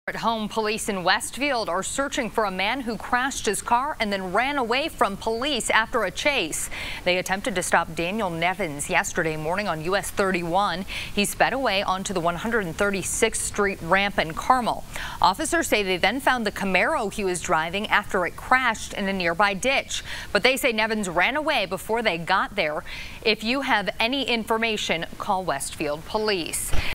Man crashes Camaro while fleeing from Westfield police, gets away on foot
A man fleeing from police near Westfield, Indiana crashed his vehicle and got away on foot early Sunday morning.